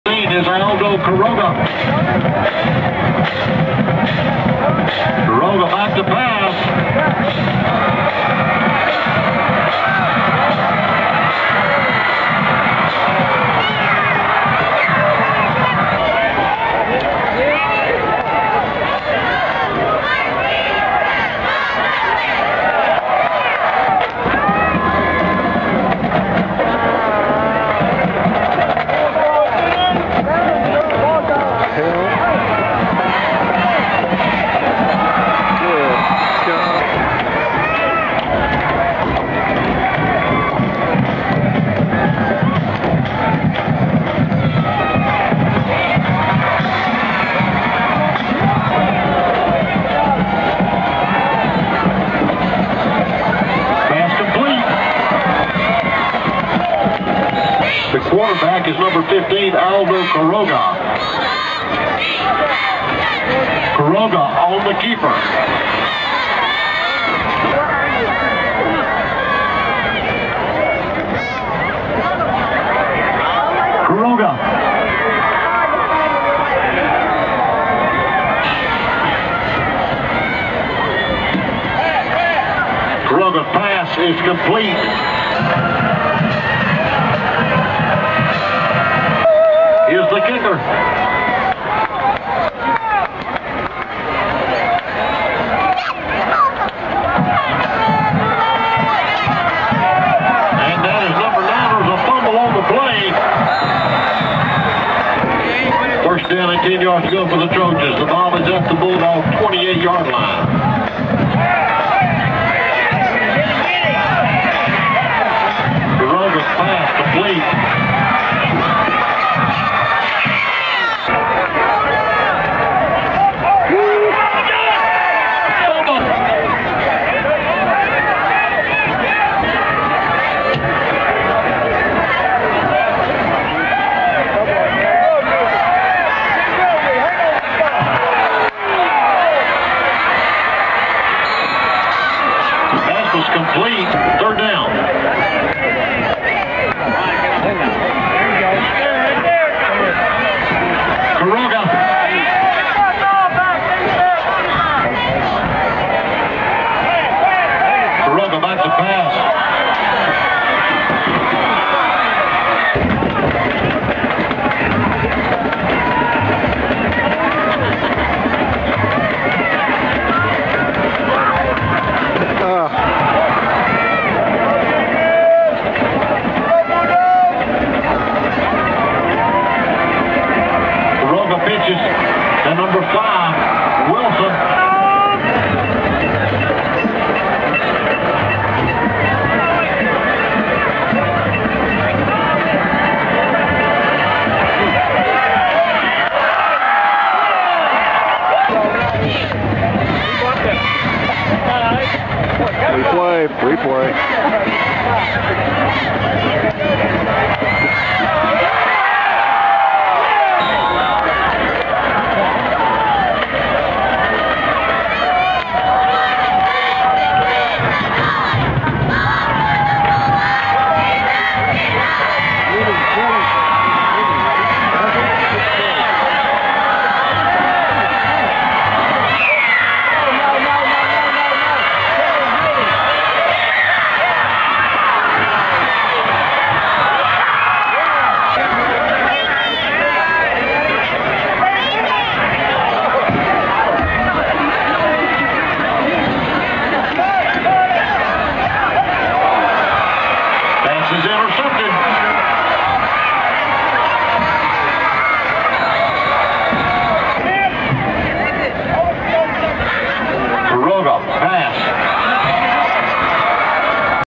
Newman Smith Royse City Video MPEG4
First Half Highlights of Newman Smith vs Royse City Texas high school football game October 30, 2009